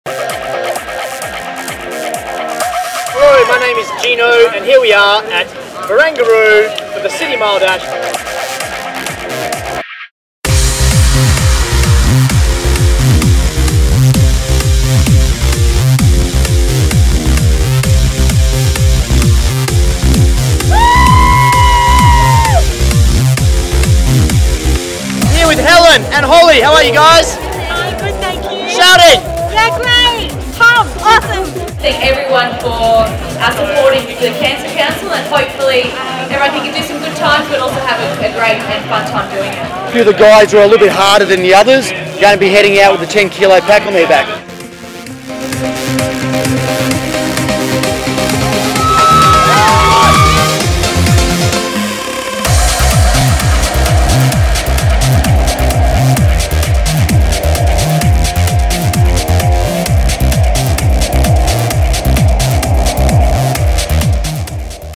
2012 Cancer Council City Mile Dash
What a day! A BIG thanks to all participants, sponsors and partners who generously supported the 2012 Cancer Council City Mile Dash at Barangaroo, Sydney. With your help we've raised over $123,000 towards ovarian and testicular cancer treatment, research and prevention programs. See you all in 2013 for another chance to tackle the one mile dash!